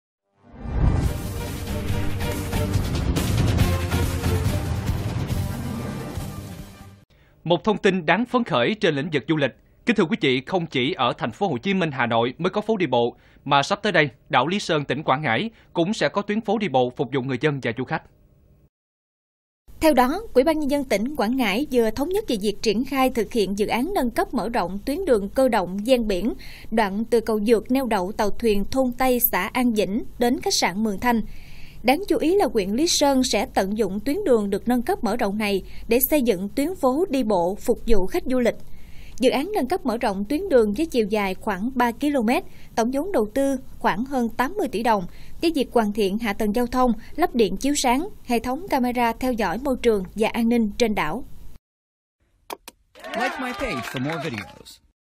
Phố đi bộ Lý Sơn
Đảo Lý Sơn sẽ có tuyến phố đi bộ phục vụ khách du lịch
Không chỉ ở TP.HCM, Hà Nội mới có phố đi bộ, mà sắp tới đây, Đảo Lý Sơn, tỉnh Quảng Ngãi cũng sẽ có tuyến phố đi bộ phục vụ người dân và du khách.